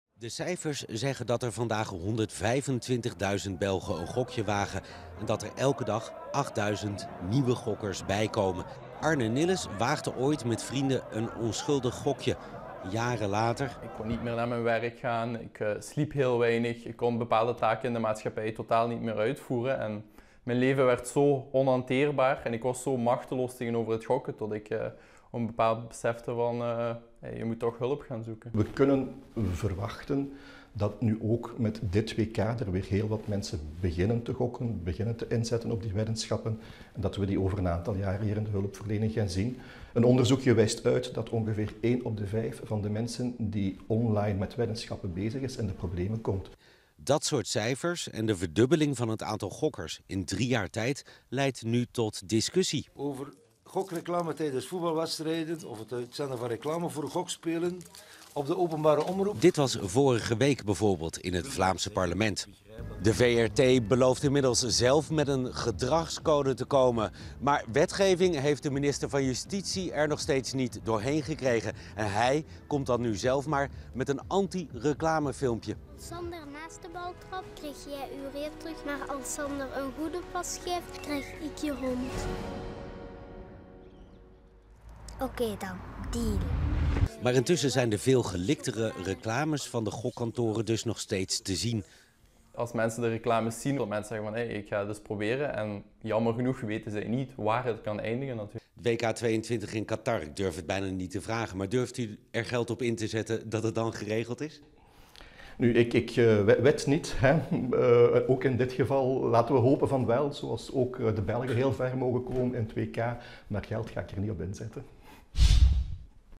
WK: Online gokken neemt flink toe in België
Al wekenlang voeren online gokmerken op de Vlaamse publieke omroep VRT uitgebreid campagne. Met veel succes, blijkt uit cijfers van de Belgische Kansspelcommissie. Waar eind mei nog dagelijks zo'n 40.000 à 50.000 mensen actief waren op de sites, zijn dat er nu meer dan 110.000. En elke dag registreren zich zo'n 8000 nieuwe gokkers, meer dan vier keer zoveel als normaal.

"Vooral wanneer de Rode Duivels spelen, gaat het aantal weddenschappen exponentieel de hoogte in", merkt directeur Peter Naessens van de Kansspelcommissie. Voorlopig piekmoment was de wedstrijd van België tegen Panama, waarop ruim 123.000 unieke deelnemers geld inzetten.

► VRAGEN/TIPS/IDEEËN?